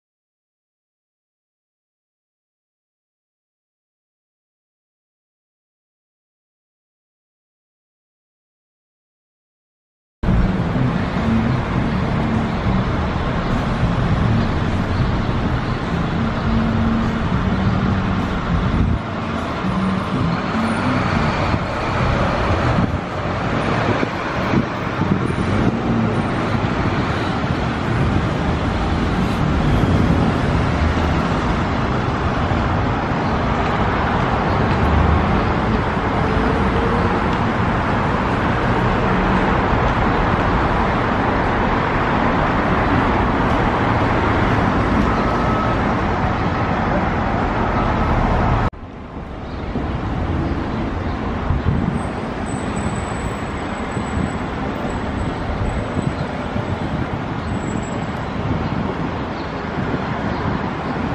Washington DC Fascinating Bus Shelter 13 June 2019
Another video in my bus shelter series. it lurks in the centre of Washington DC. a truly memorable bus shelter. It was filmed on Thursday 13 June 2019.